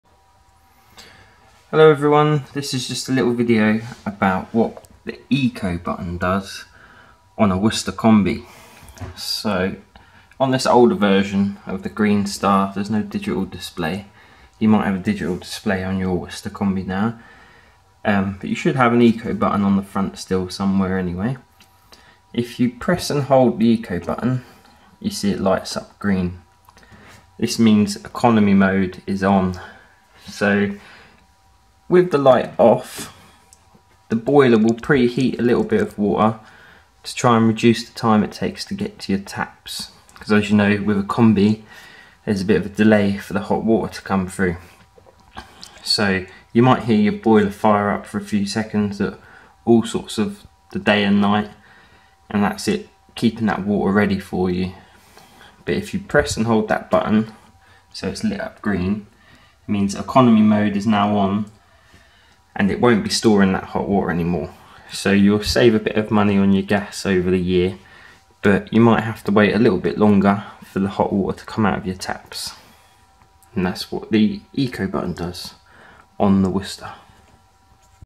Worcester Bosch ECO button - what is it and what does it do?
The worcester combination boilers have a preheat function that you can turn off and on with the ECO button.
When the ECO mode is on the boiler will no longer preheat and it may take longer for the water from your taps to run hot, however you will save money on your gas.
If ECO is off and preheat is on then the boiler will fire up for a few seconds at random times of the day and night.